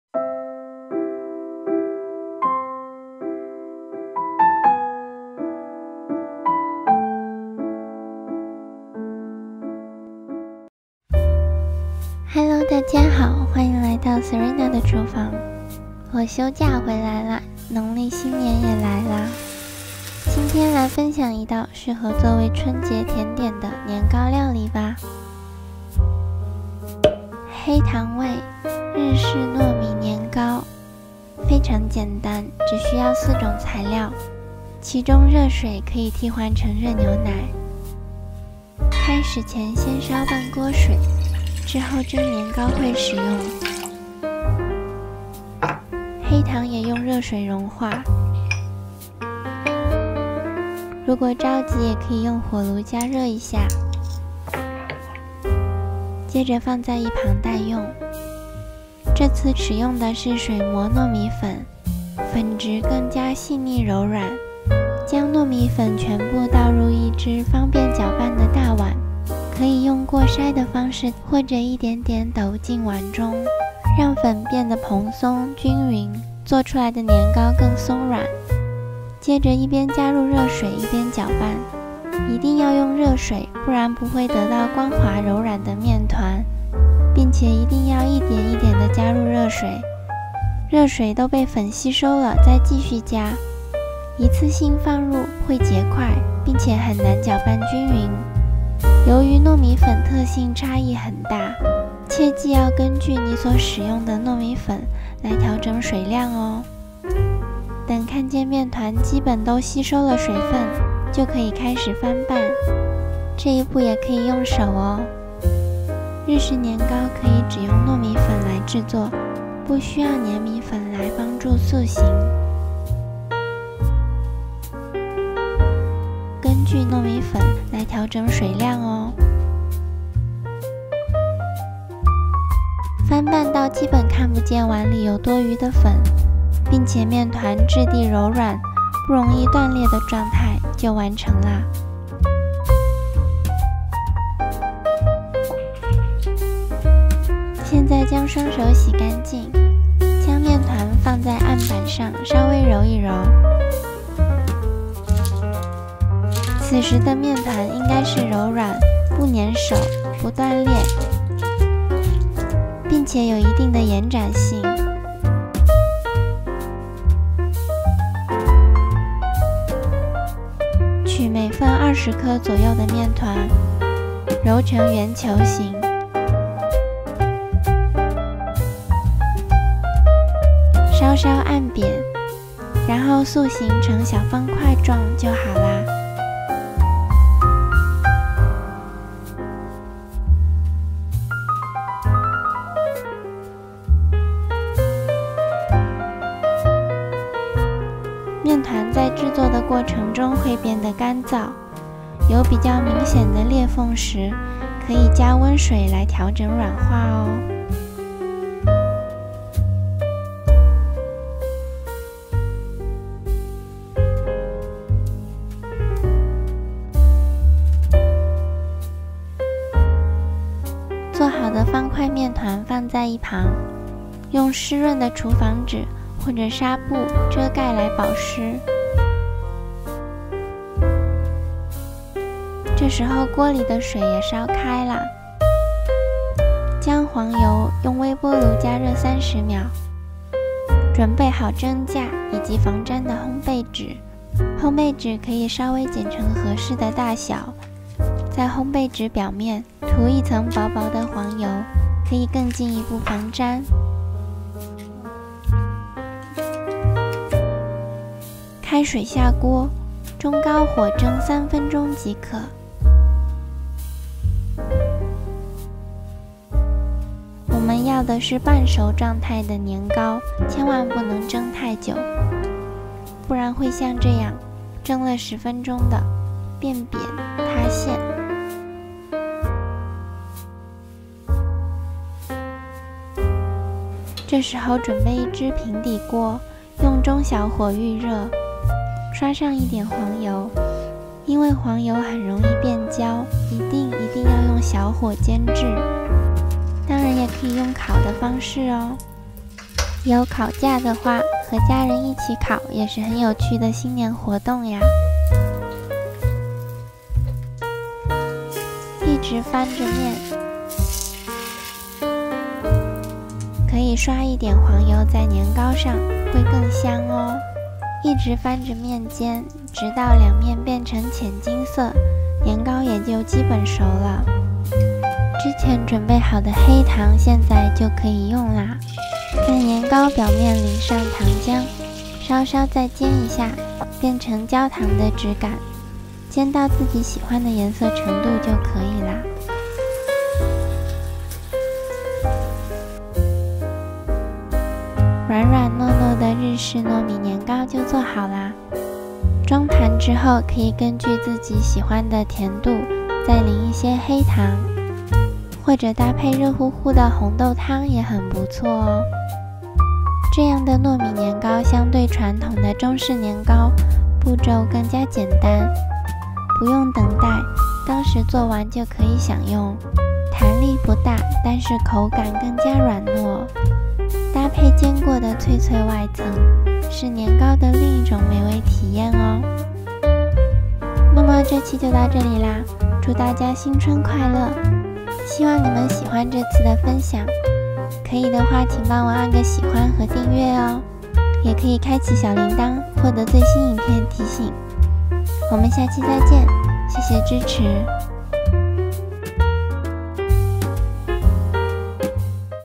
Serenade Cuisine | 黑糖味 日式糯米年糕 Brown Sugar Japanese Rice Cake (Tutorial)
軟糯可愛的年糕~
你要不要嘗一口？
Soft and cute rice cake~
you wanna try?